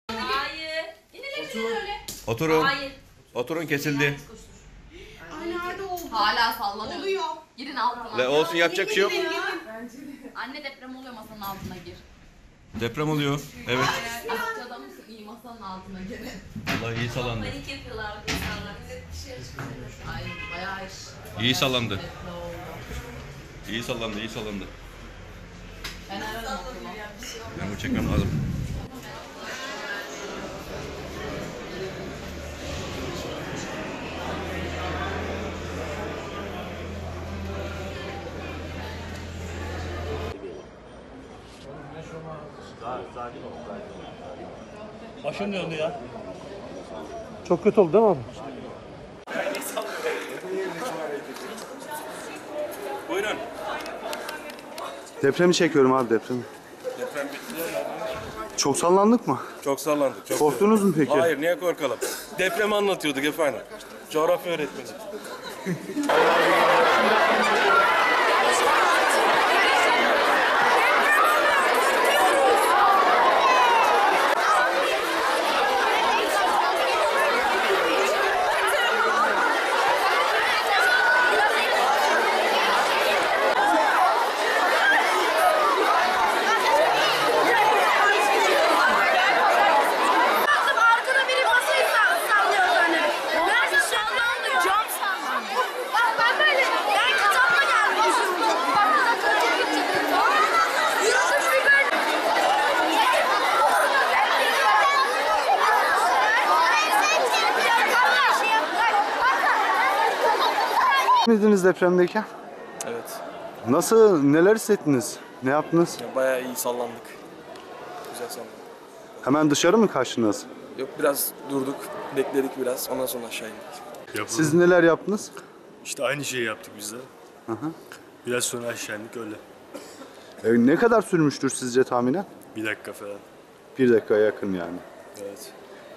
Denizli'de Deprem Oldu, Vatandaşlar Büyük Panik Yaşadı
Denizli’de 5.5 şiddetinde meydan gelen deprem büyük paniğe neden oldu.

Deprem merkez üssünün Acıpayam ilçesi olduğu belirtildi. 5.5 şiddetindeki deprem yaklaşık olarak 20 saniye sürdü ve büyük panik yaşattı. Depremim hemen ardından okullarda korku dolu anlar yaşanırken sınıflar boşaltılarak öğrenciler okul bahçelerinde güvenli alanlara toplandı. Bazı küçük yaştaki öğrencilerin ise yaşadıkları korkudan dolayı ağladıkları gözlendi.
Depremde yıkım bilgisi ölen ya da yaralanın olup olmadığı ile ilgili açıklama yapılmadı.
Depremin merkez üssü Acıpayam’da ise vatandaşlar sokaklara döküldü.
Öte yandan özel bir lisede Coğrafya öğretmeni olarak görev yapan bir şahıs ise derste deprem konusunu işlerken deprem olduğunu ve hemen dışarı çıktıklarını belirtti.